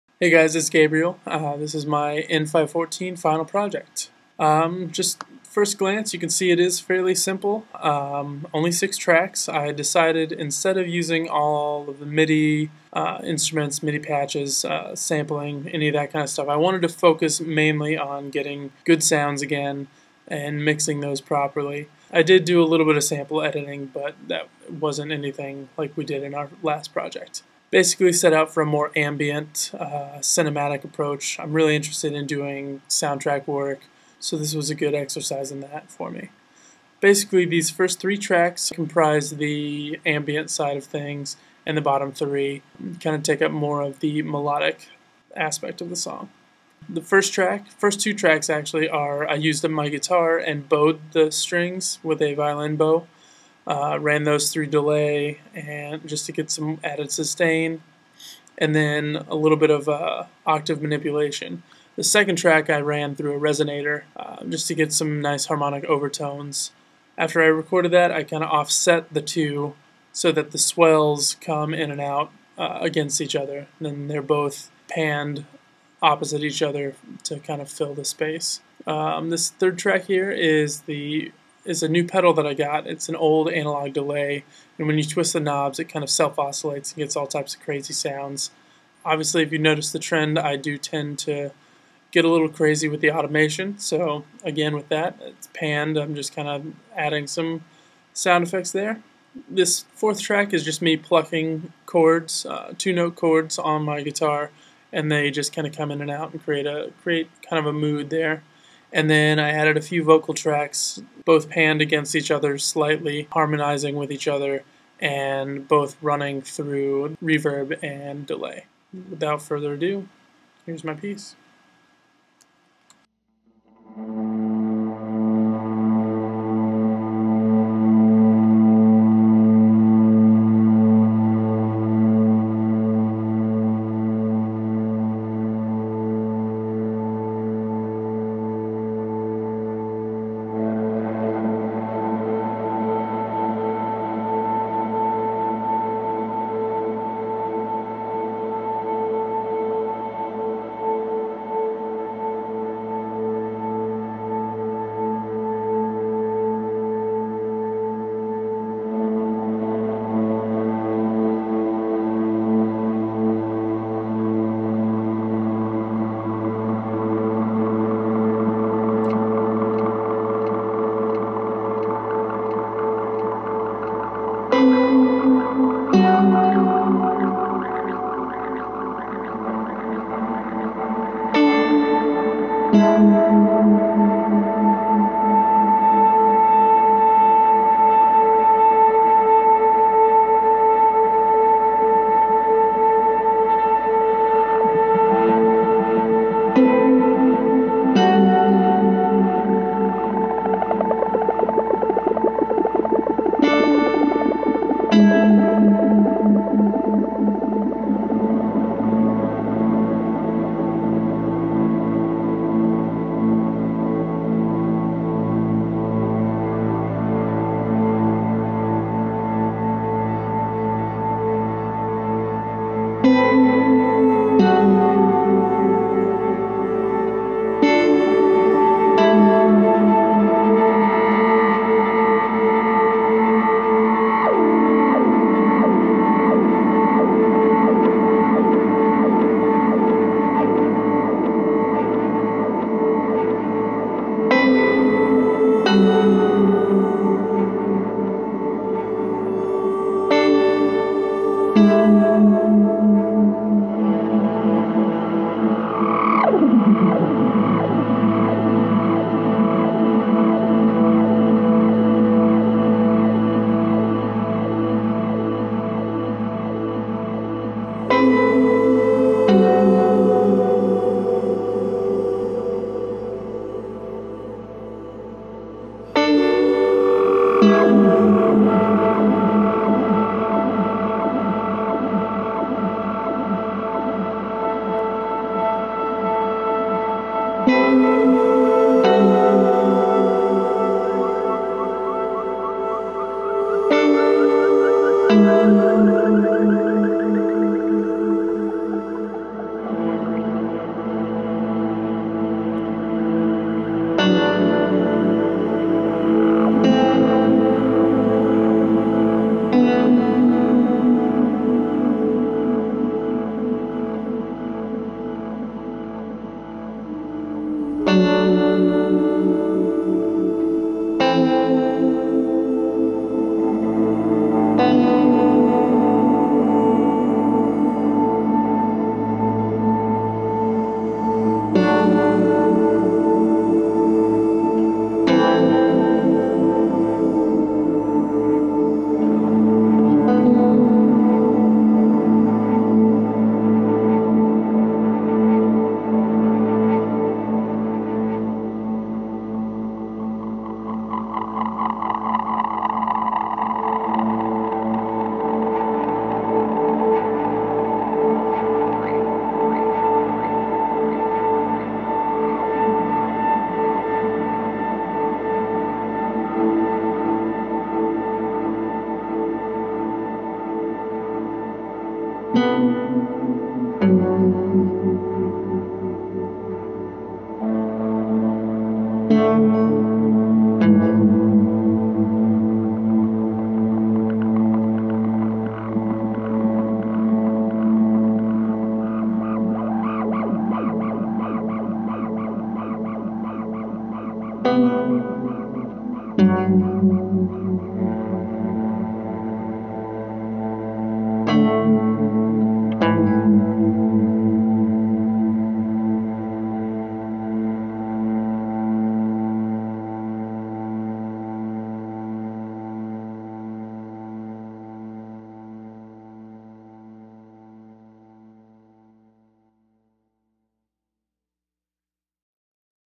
Garber-FinalProject-042512
Original composition by Gabriel Lee Garber, composed for N514 at IUPUI.